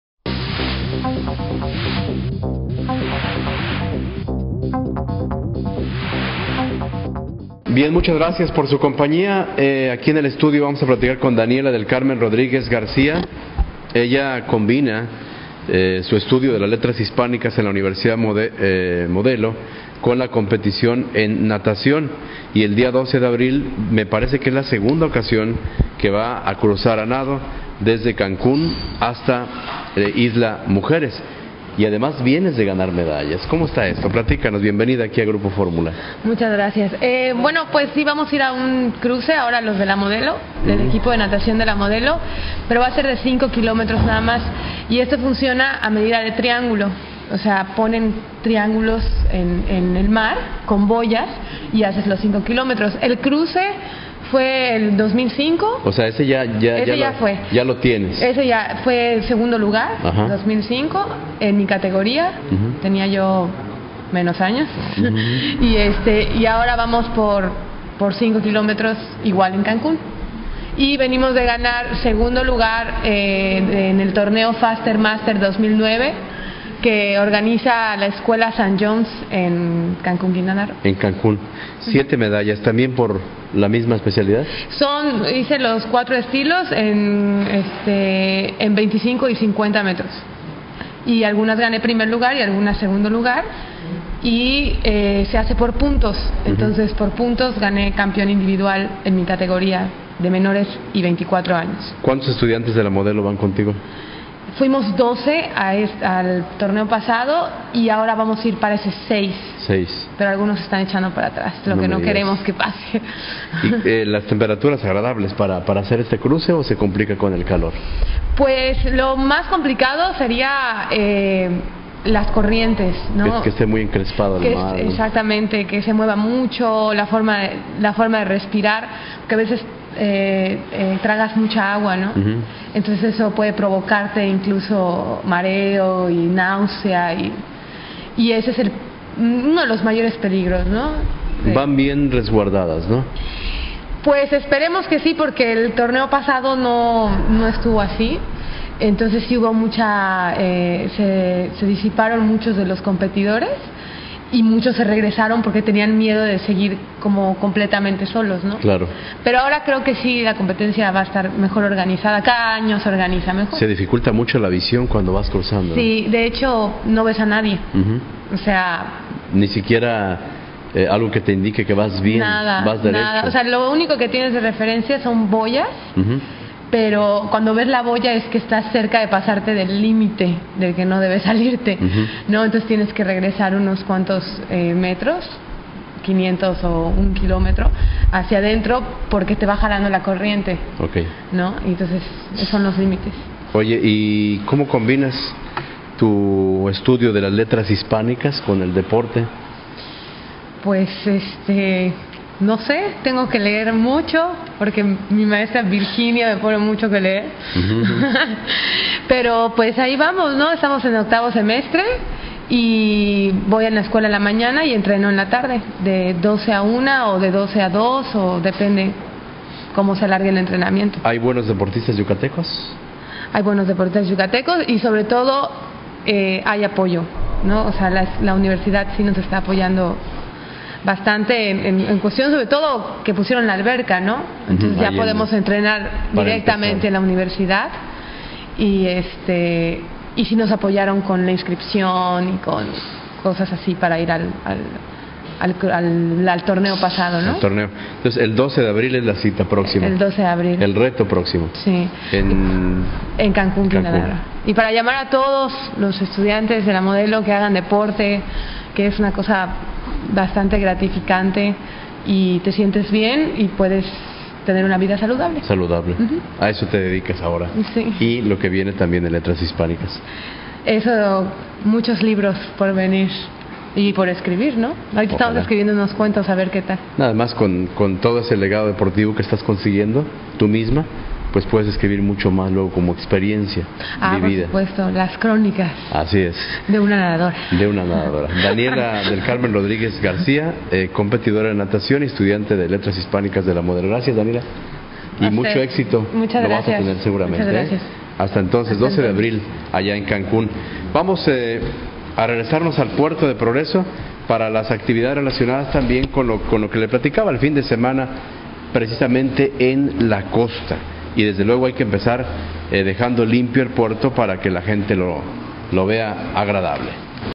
Yucateca cruzará a nado Cancun-Isla Mujeres
Mérida Yucatán "Noticias Mérida" "Revista Yucatán" "Jose Luis Preciado"